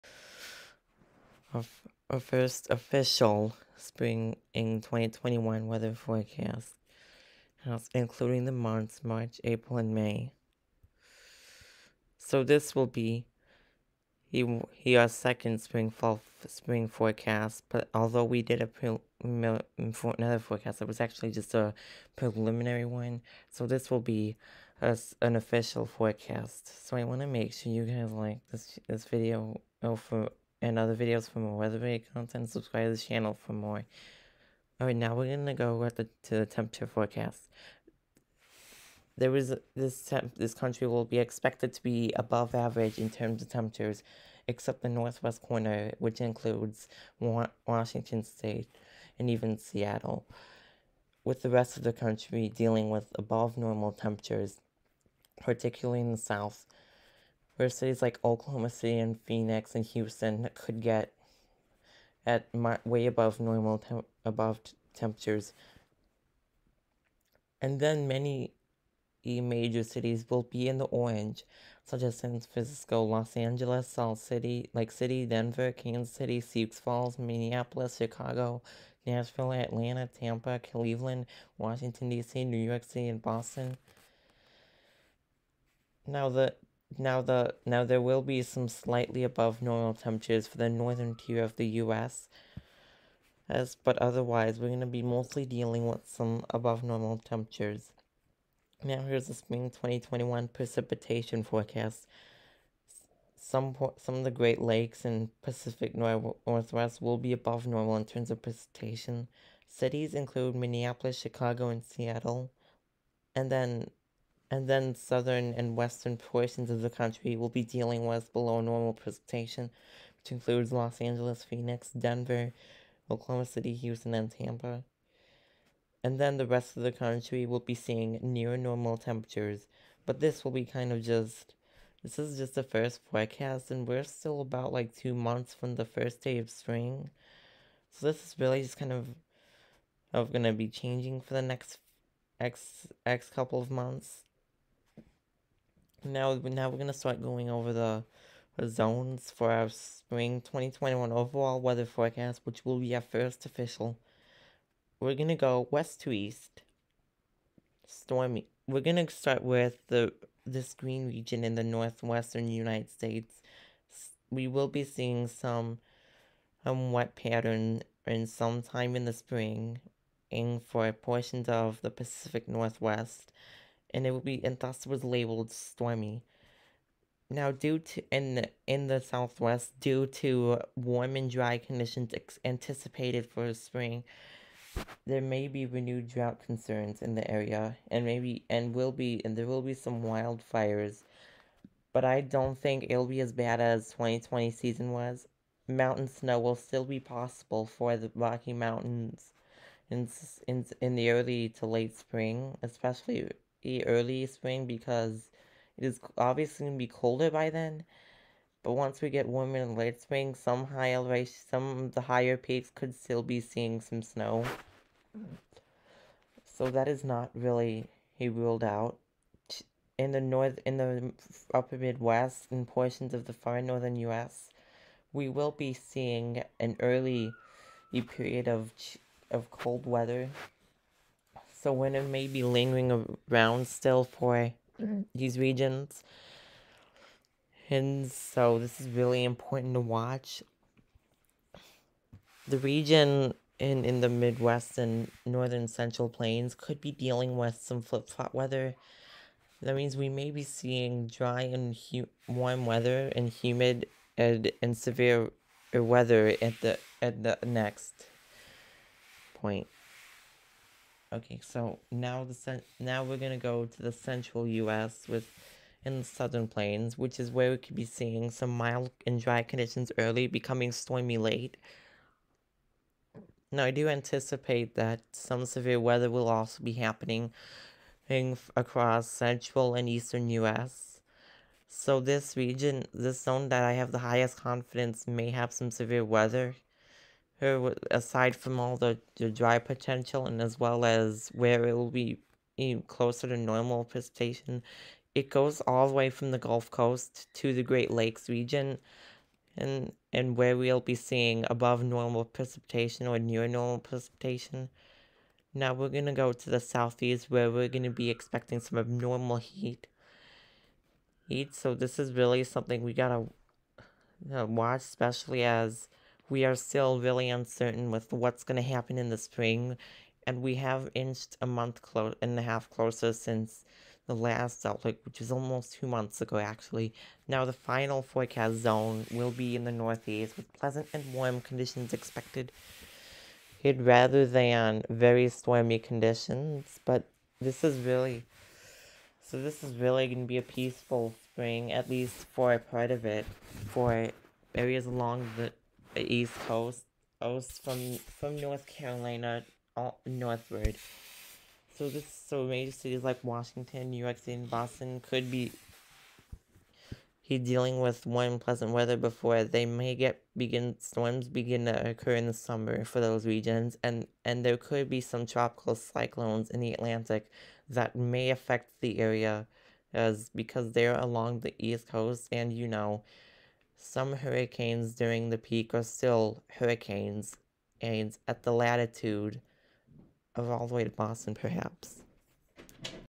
Official Spring 2021 Weather Forecast!
We will be going over what to expect in our next season as spring continues to inch closer every day, we will talk about how the temperature and precipitation will shape our spring, this will be a wet and stormy one for Great Lakes.. while the South may see tornado outbreaks. Here's to what may come this spring, a more detailed forecast will be released on Feb 16.

More forecasts to come:
Feb 1: February 2021 Monthly Forecast
Feb 4: 2021 Atlantic Hurricane Season Forecast
Feb 12: 2021 Eastern Pacific Hurricane Season Forecast